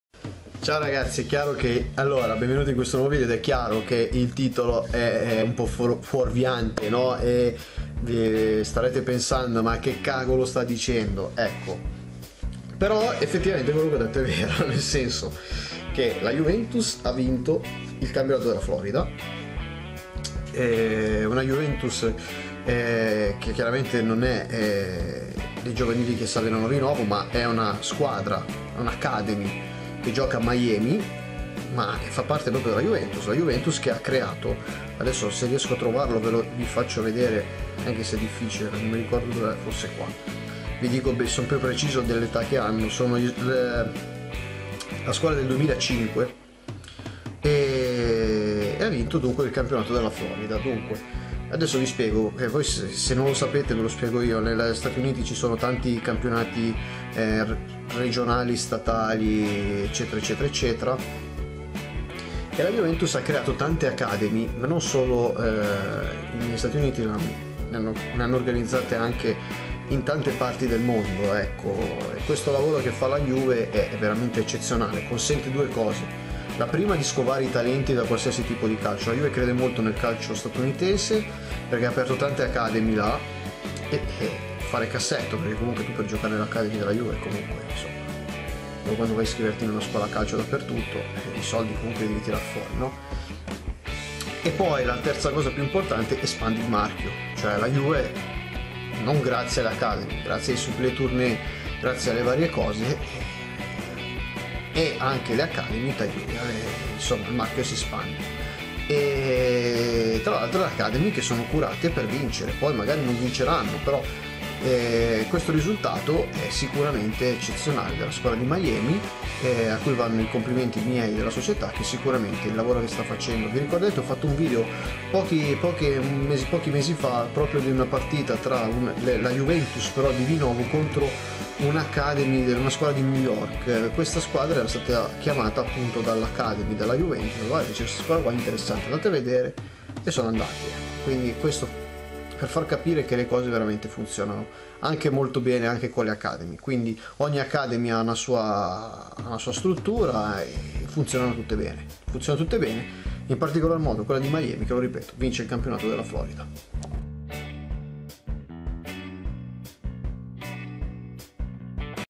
LA JUVENTUS E' CAMPIONE DELLA FLORIDA
la under 13 della Academy di Miami porta a casa il titolo dimostrando l ottimo lavoro delle academy e del settore giovanile